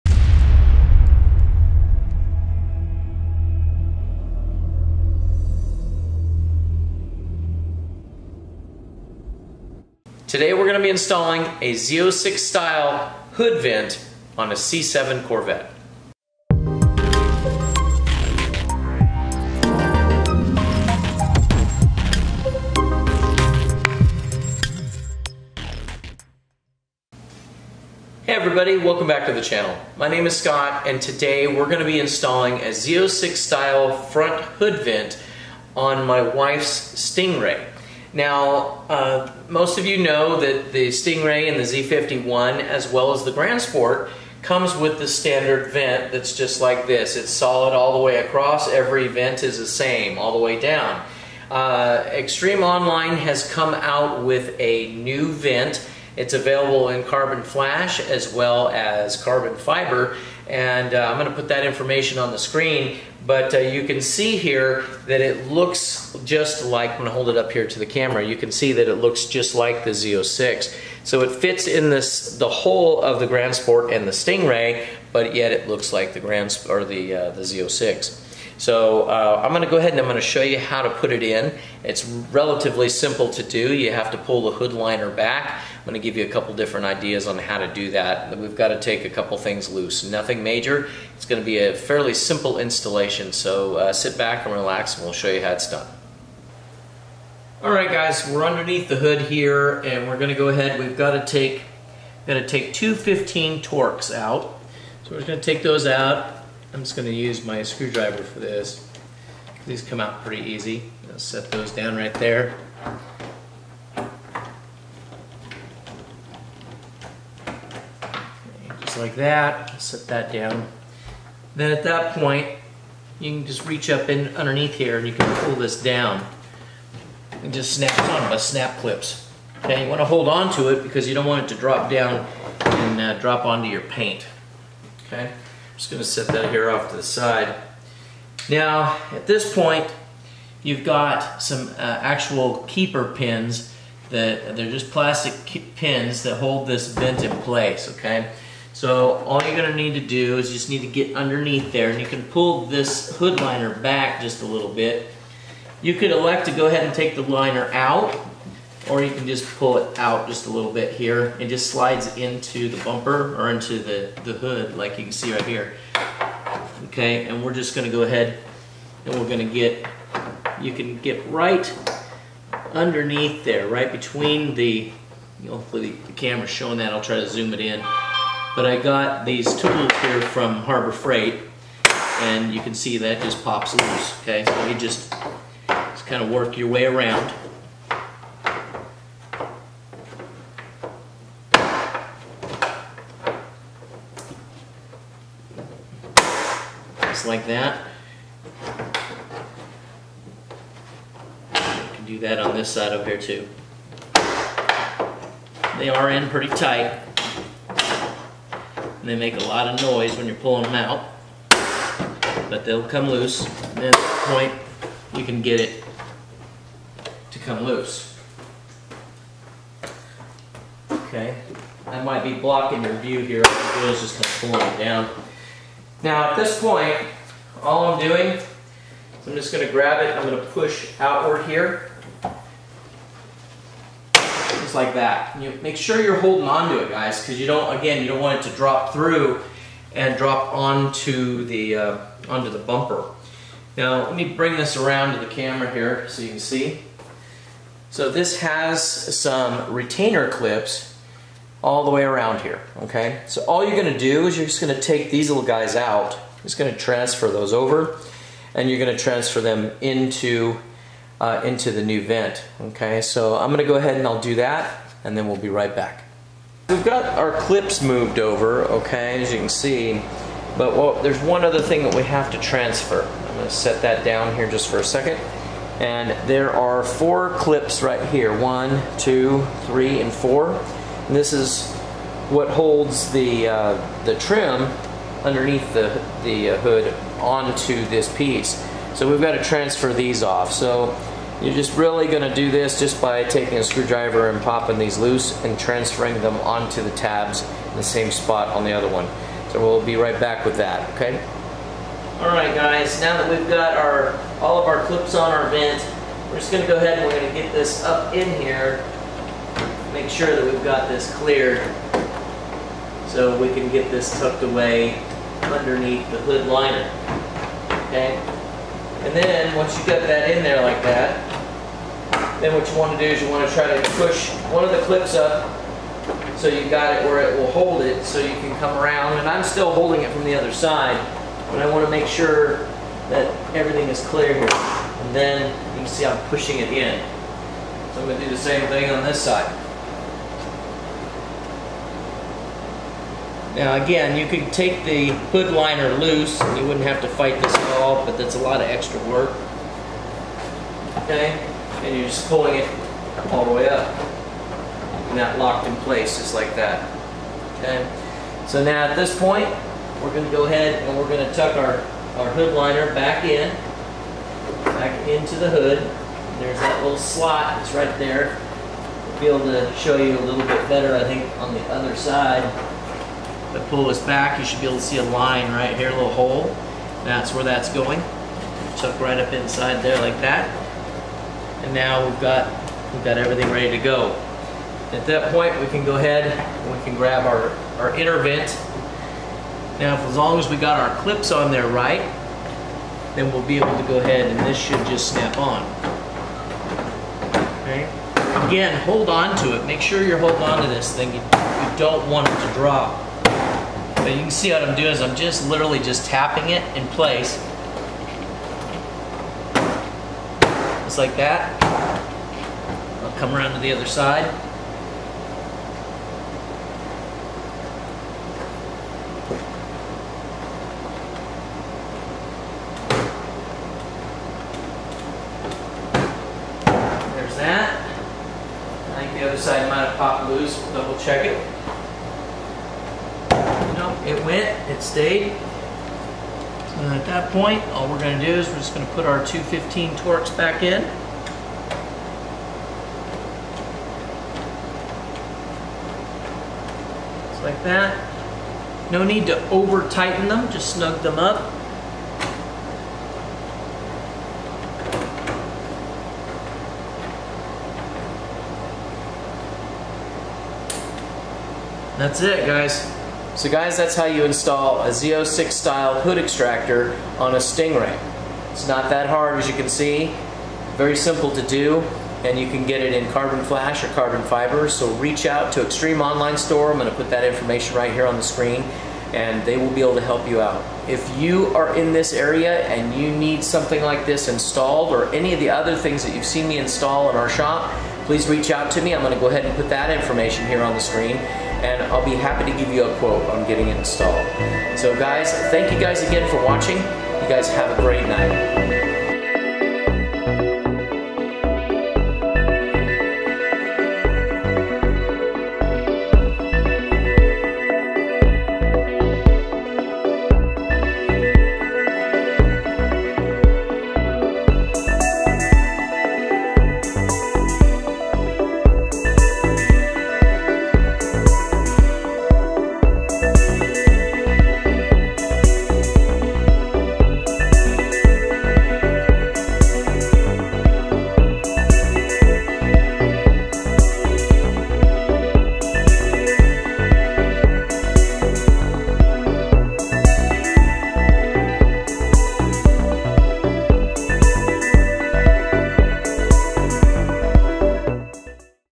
How to Install a Z06 Style Hood Vent on a C7 Stingray or Grand Sport
In Todays Video we show you How to Install a Z06 Style Hood Vent on a C7 Stingray or Grand Sport